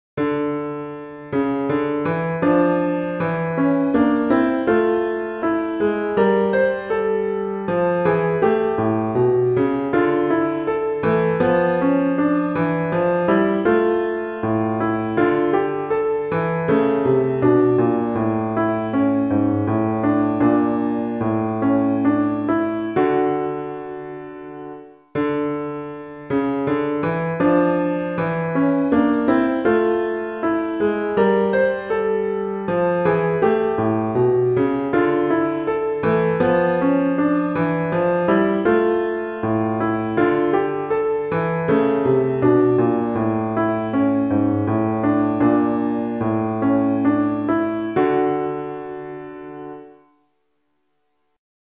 Análisis de un Canon a la octava superior a dos voces para piano. En modo menor
En la partitura se indican todos los aspectos armónicos, acordes, grados y notas de adorno: notas de paso, floreos, retardos, escapadas y anticipaciones.